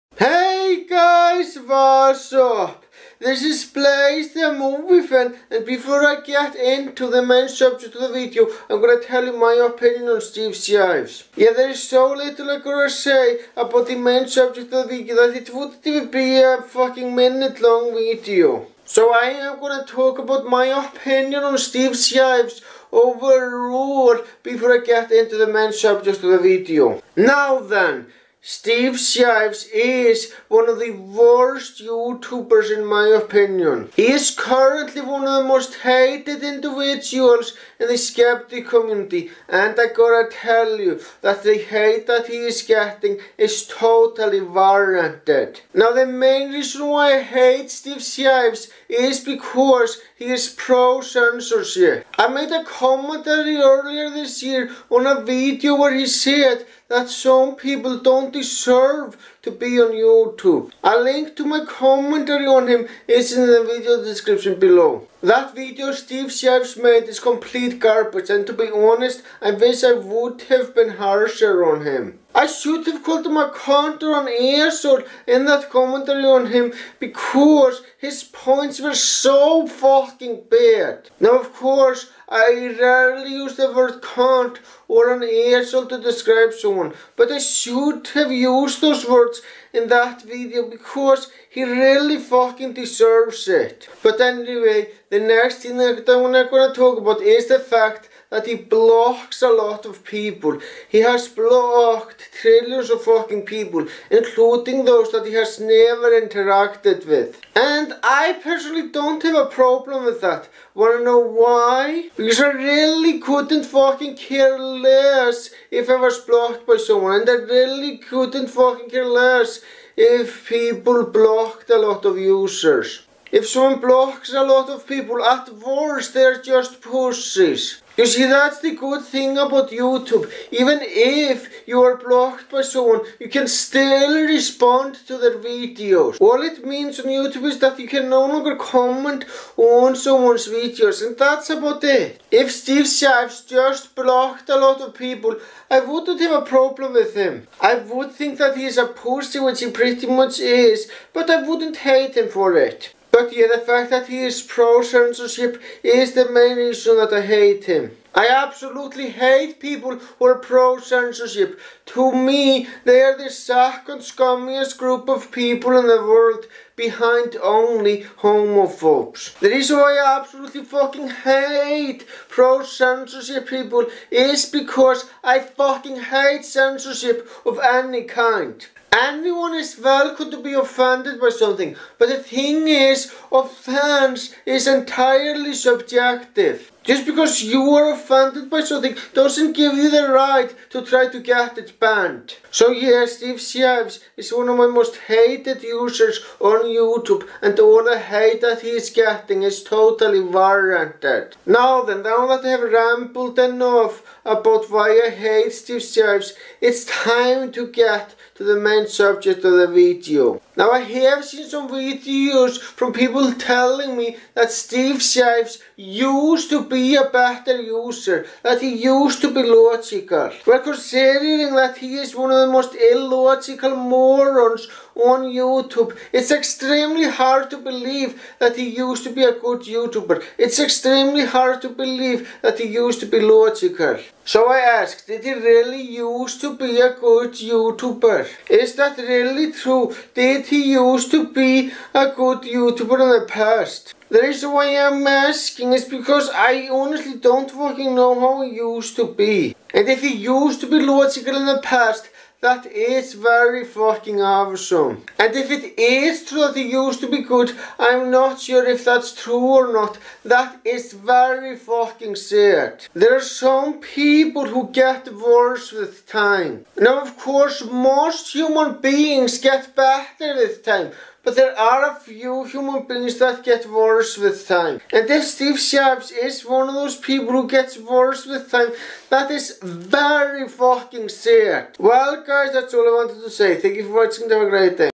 Did Steve Shives really use to be a good YouTuber?
I would link the video where someone mentioned that Steve used to be a good user, but the problem is I don't remember what that video was called.